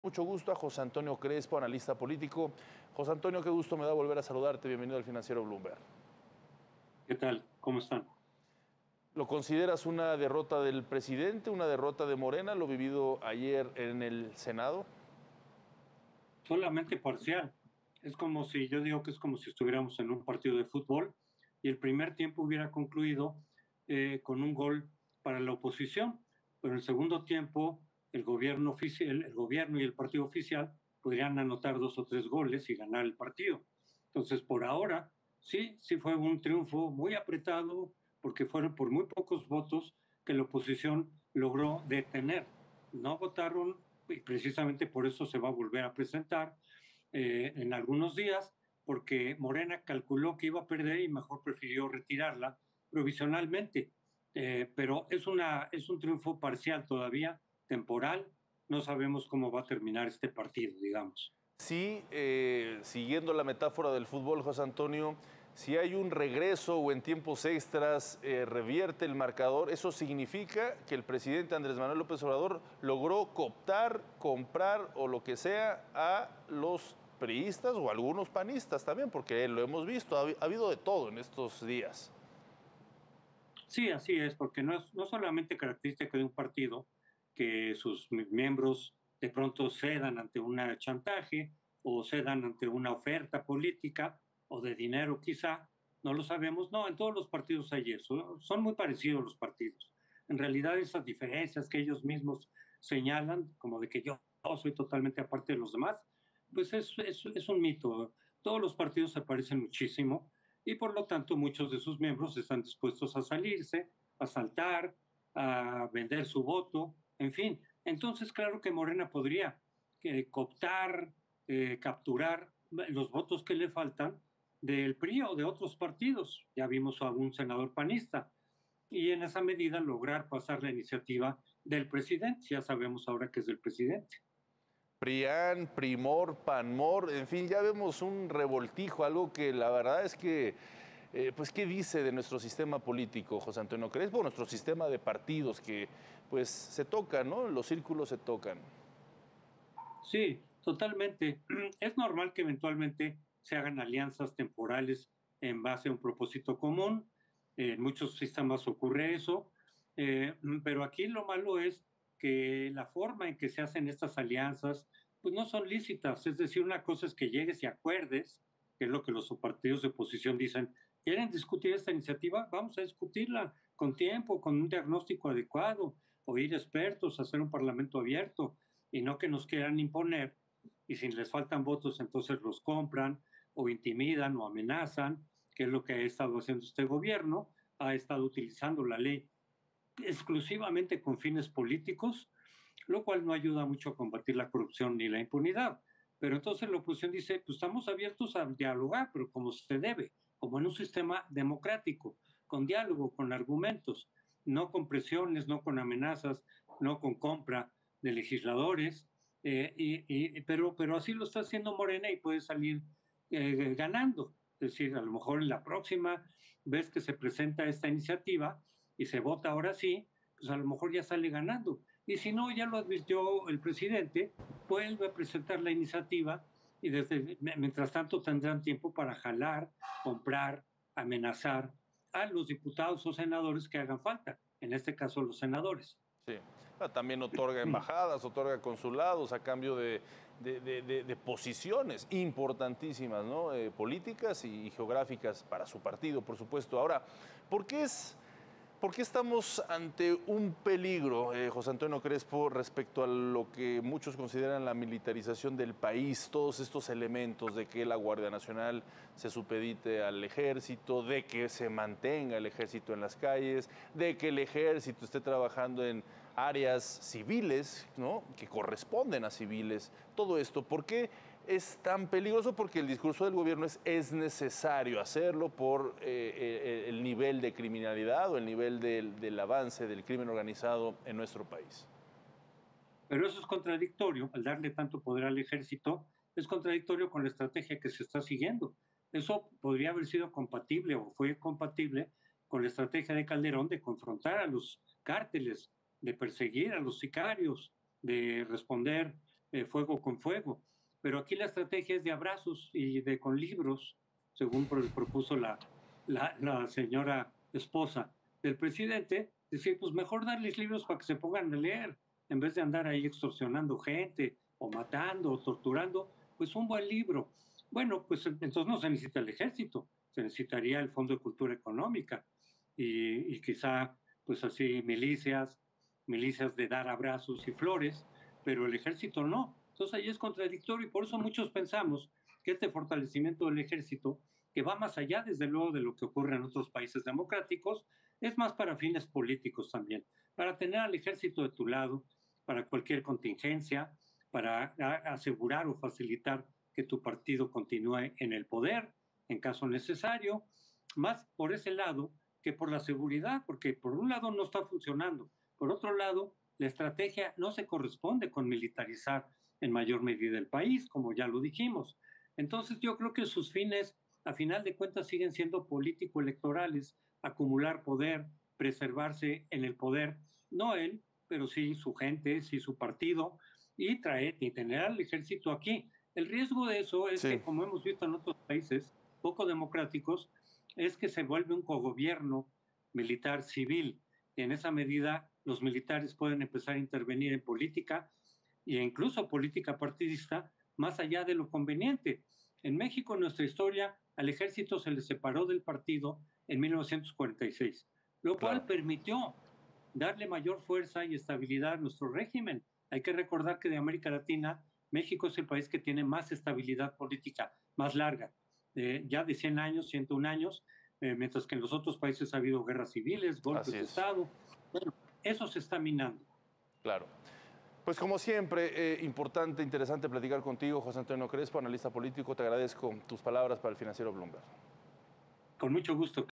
A los de Morena los DERROTARON parcialmente: José Antonio Crespo
El analista José Antonio Crespo dijo que la oposición derrotó a Morena en el Senado al no aprobar la iniciativa que implicaría extender la presencia del Ejército en las calles y agregó que todos los partidos son lo mismo.